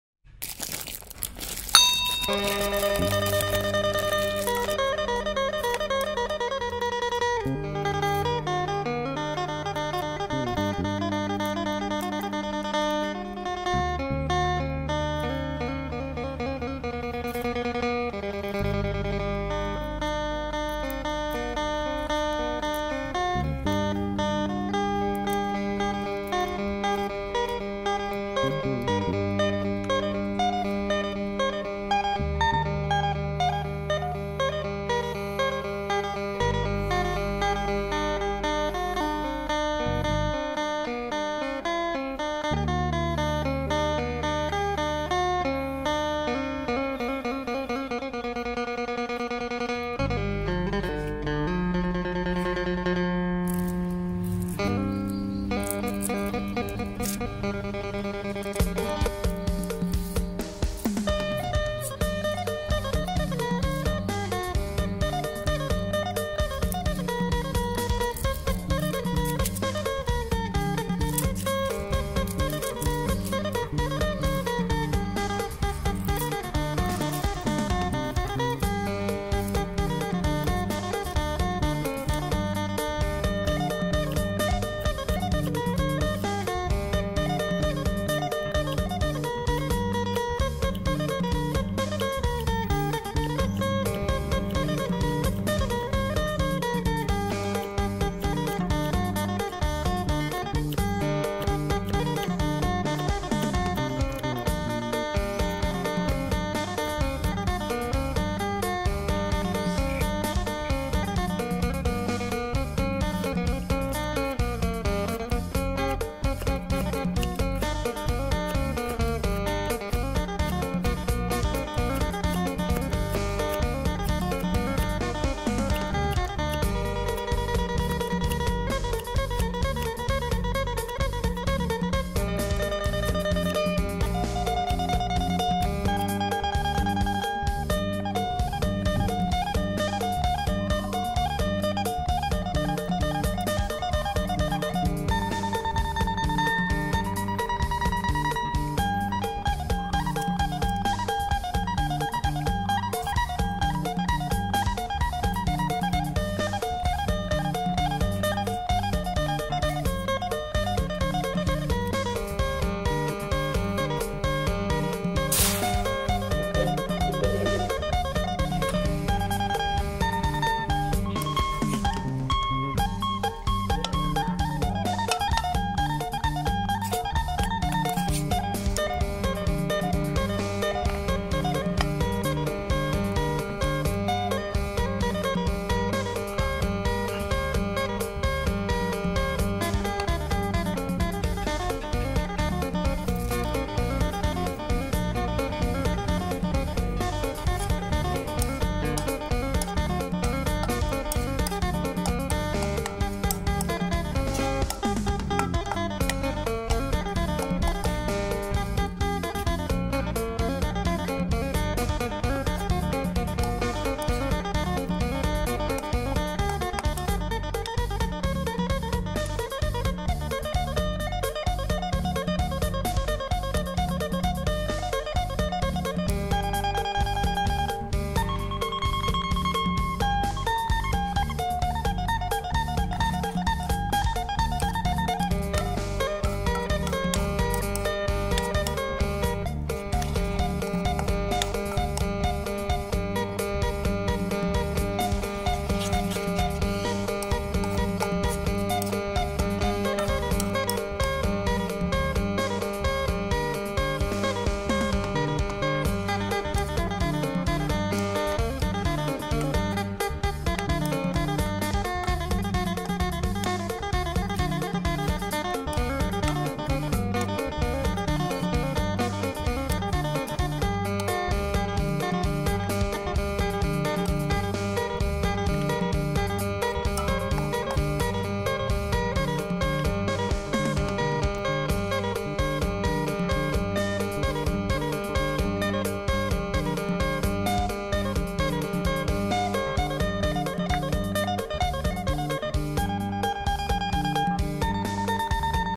Shrimp​ Sauce​ in​ Coconut​ milk/Shrimp dip in coconut milk/Shrimp in coconut sauce |Mini Food Yummy
Shrimp sauce in coconut milk is another kind of dipping sauce.
The main ingredients are shrimp, herbs, and coconut milk.
The flavor is sweet, salt and umami. Usually, serve with fresh vegetables.